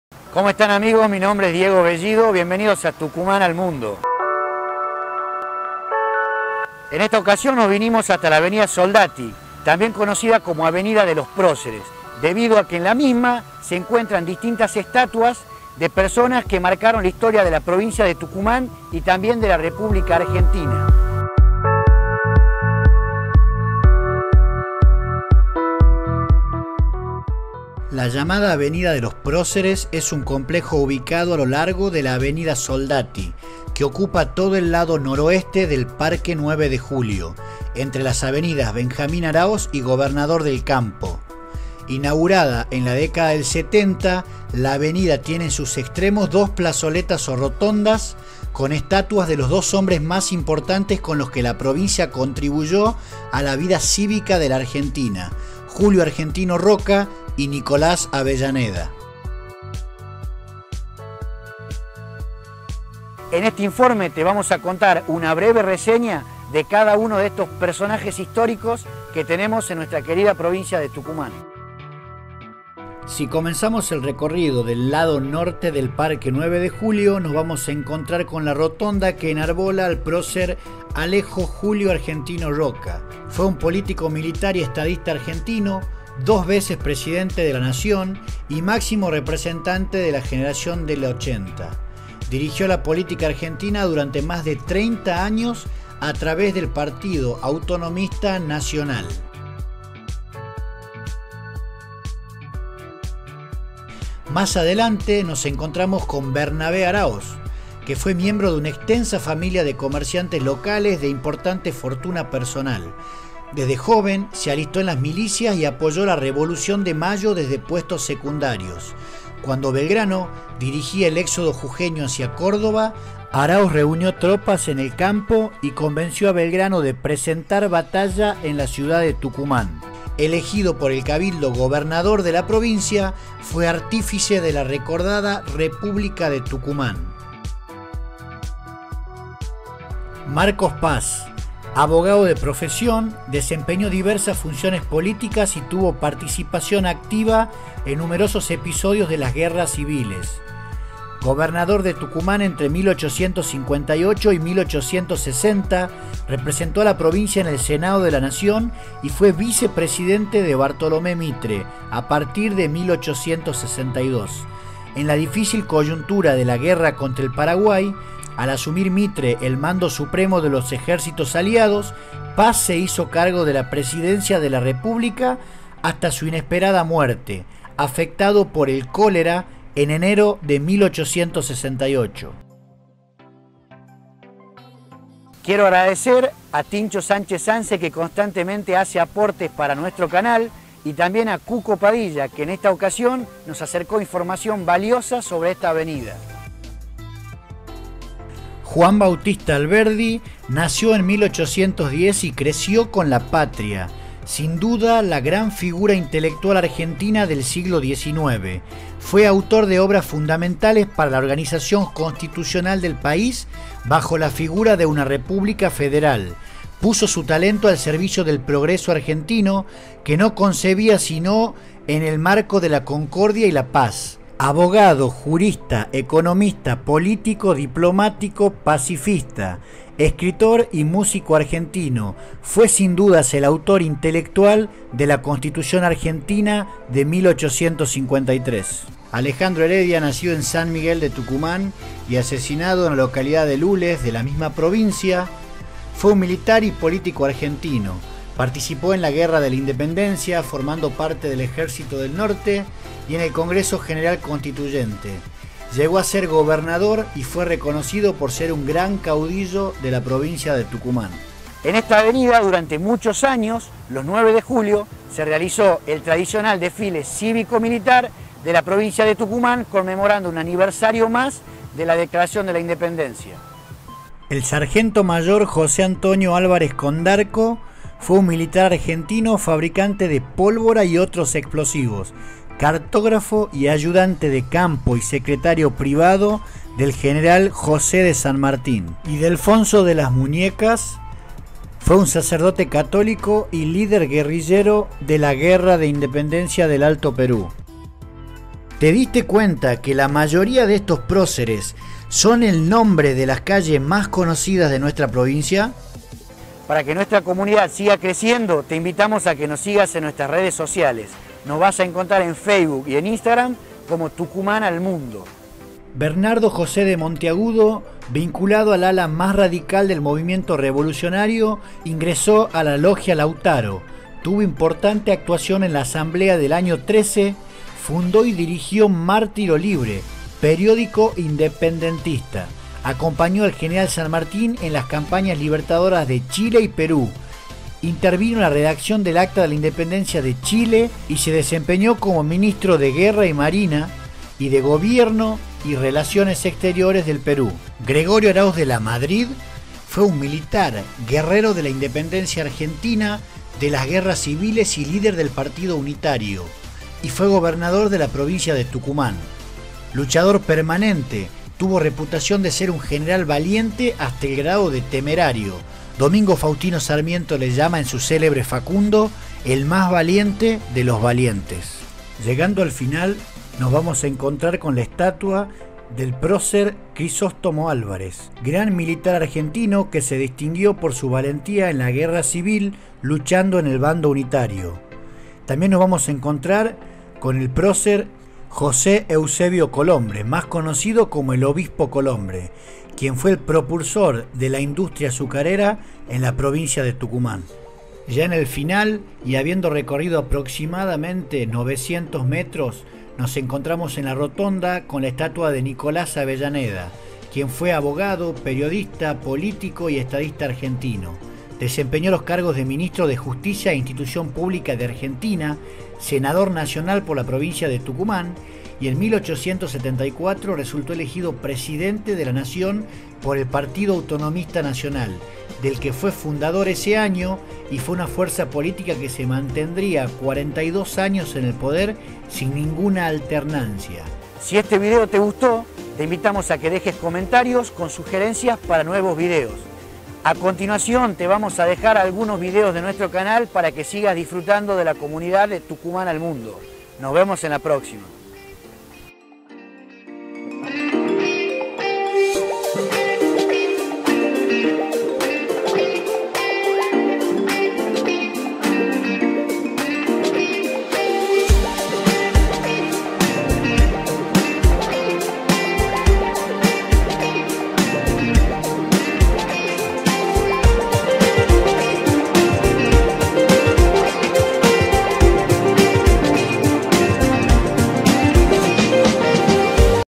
Avenida de los Próceres PARQUE 9 DE JULIO Tucumán
Enterate uno a uno quienes son estos personajes historicos que enarbolan la avenida Soldatti de la Provincia de Tucumán

EN NUESTRO CANAL ENCONTRARAS TODO SOBRE TUCUMAN.